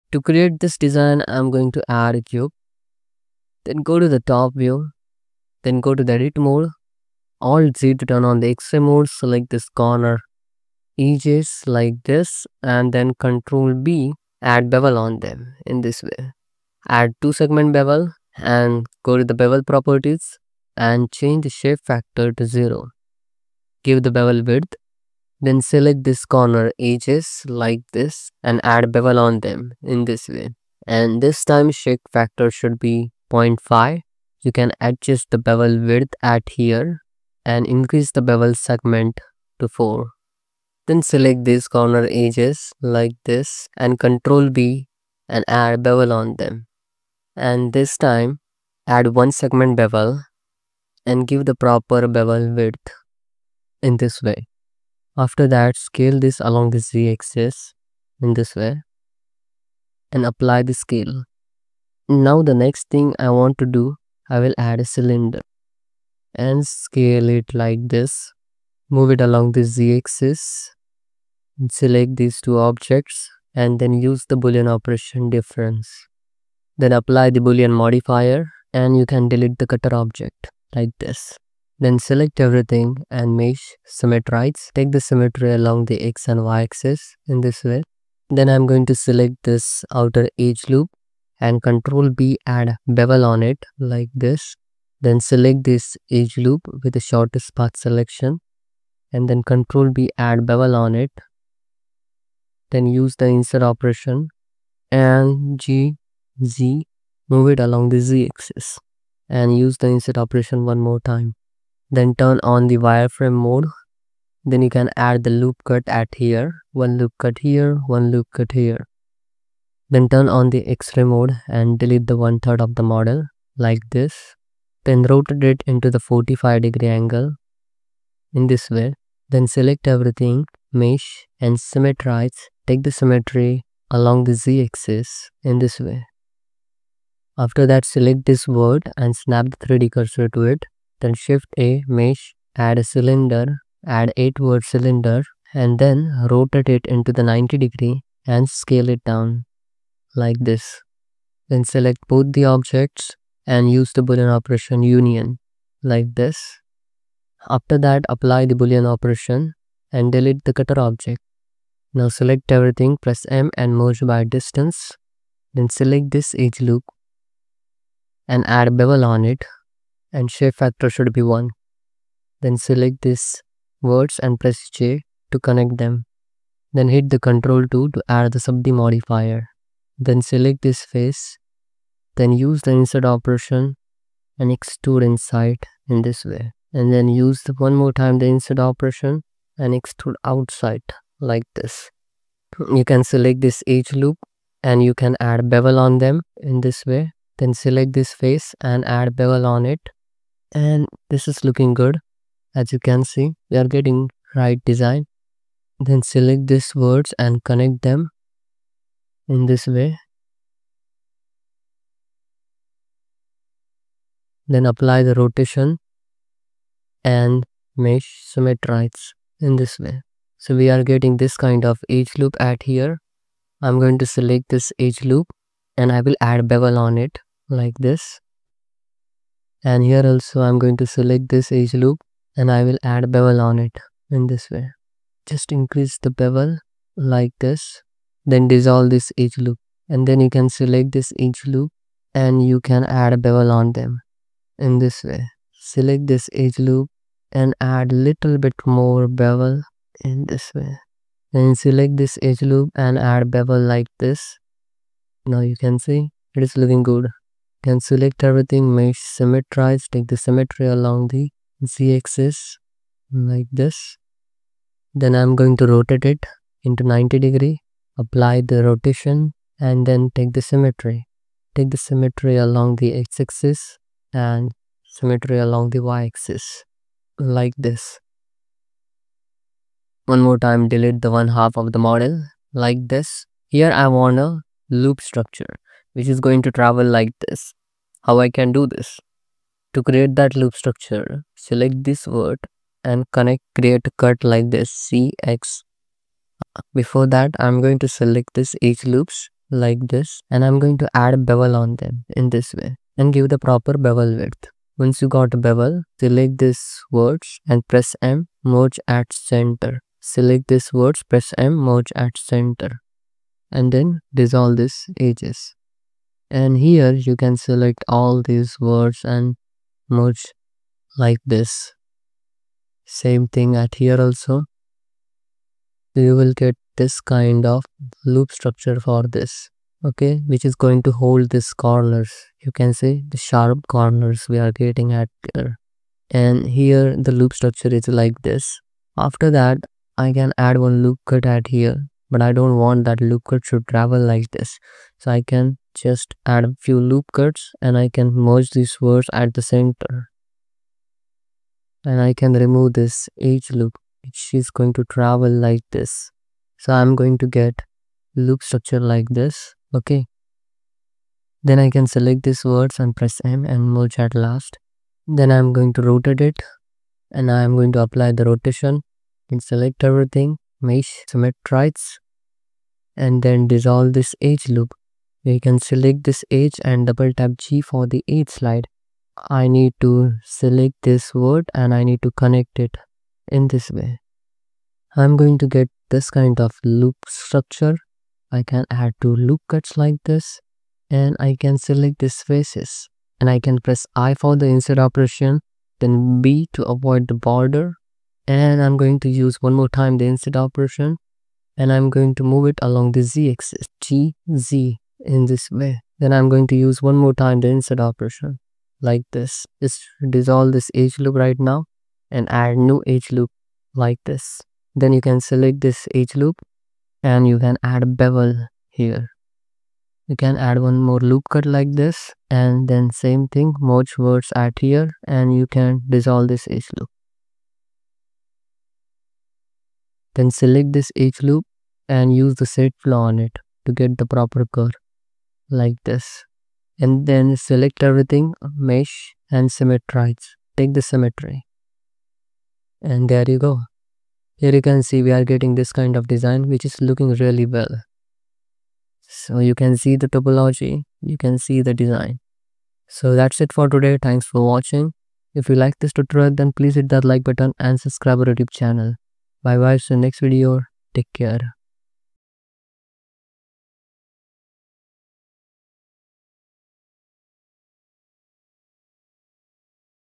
NH73 _ how to model this hard surface design _ blender modeling tutorial
In this blender tutorial I have done blender modeling to model this hard surface design. This blender 3d modeling tutorial will help you to improve your 3d modeling skill. I hope you will like this tutorial.

blender modeling NH73
hard surface prop modeling in blender
blender hard surface prop modeling
blender modeling

ABOUT US